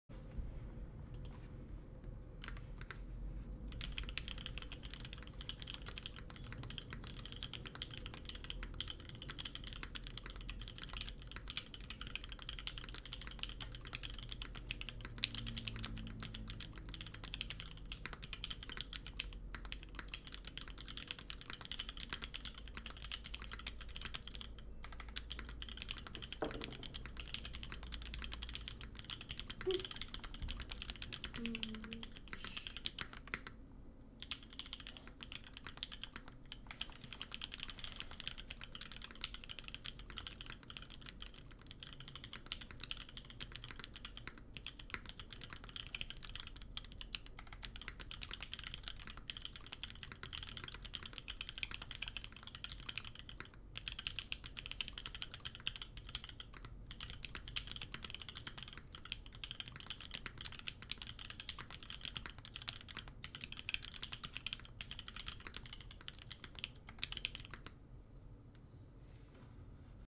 Late night typing - Episode 1: Keycult 1/65 Rev 1 with Alpaca switches
Keyboard specifications:

- Case: Keycult 1/65 Rev 1
- Plate: Polycarbonate
- Switches: Alpacas
- Springs:  62.5 Sprit
- Lube: Krytox 205g0
- Stabilizers: Durock
- Keycaps: OG Doubleshot